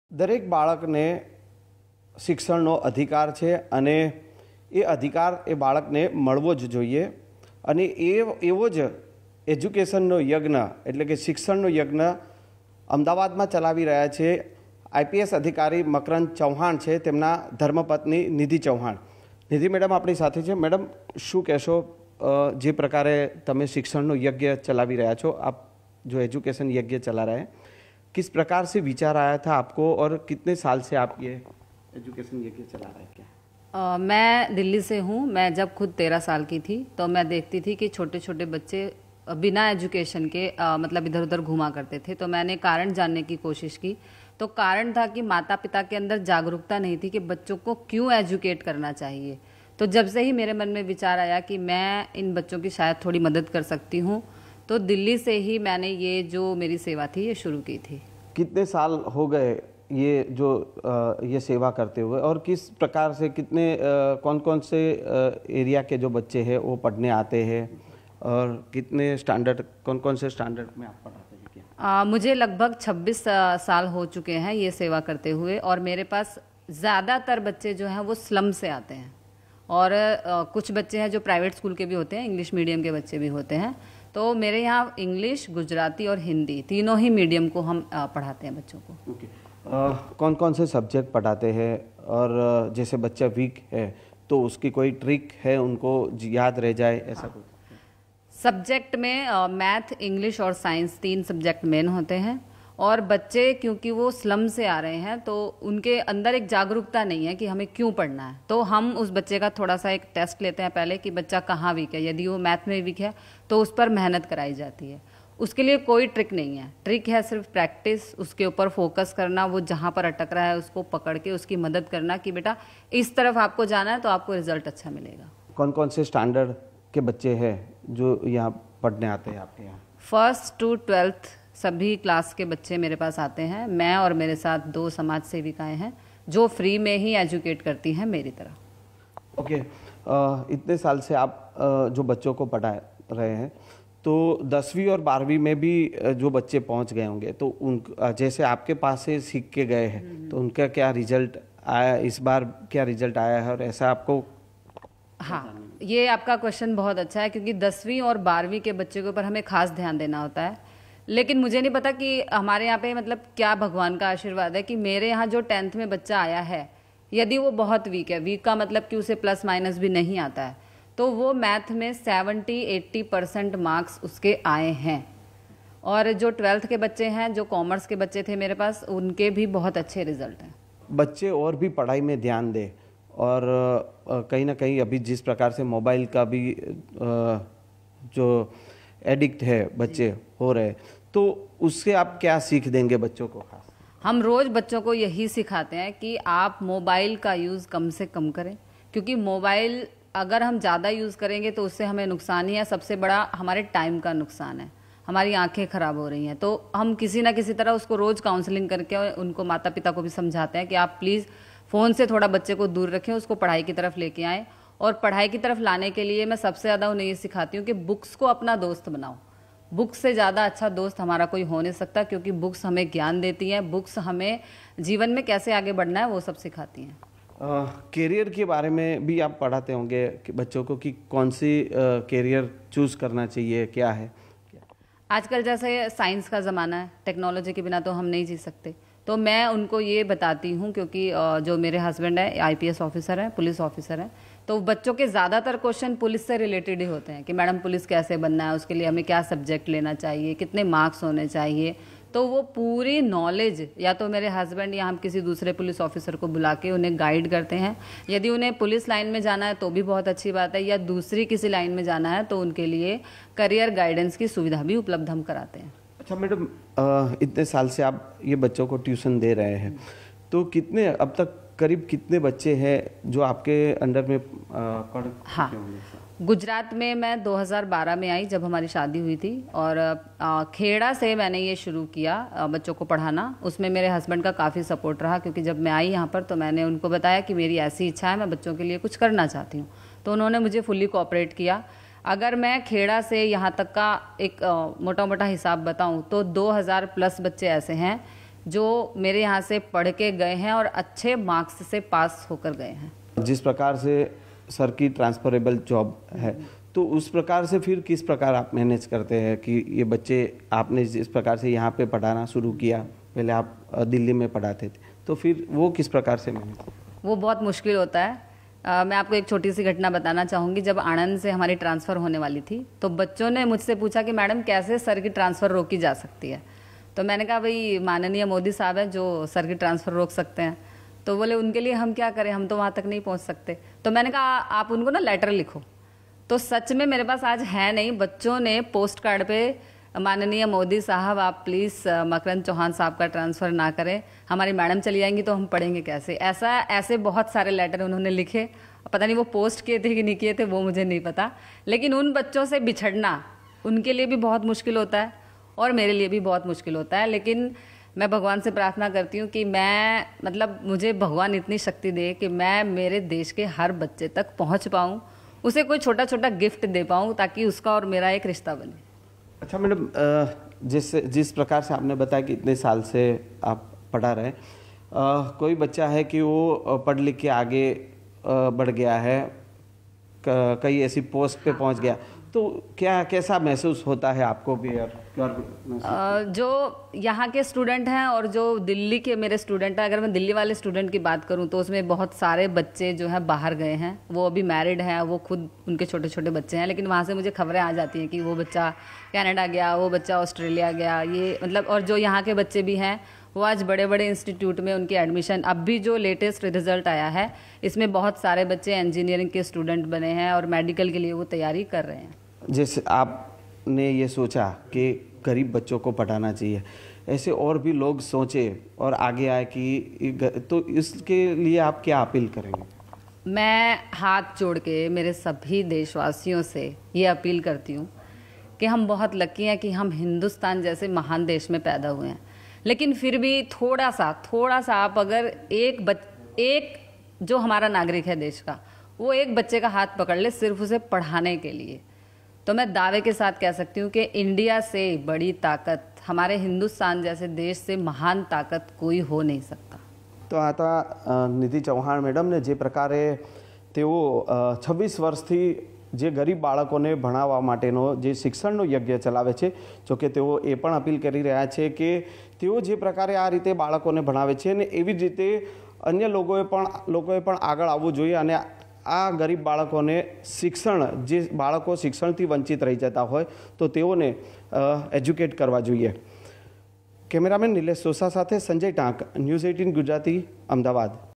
Ahmedabad News : 26 વર્ષથી ગરીબ બાળકો માટે IPSના પત્નીનો શિક્ષણ યજ્ઞ | IPS | Nidhi Chauhan
For 26 years, Nidhi Chauhan has devoted herself to the noble cause of providing free education to underprivileged children. As the wife of an IPS officer, she has utilized her position and resources to uplift the lives of children living in slums. Nidhi's unwavering commitment and advocacy have given countless young minds the opportunity to learn and thrive, despite their challenging circumstances.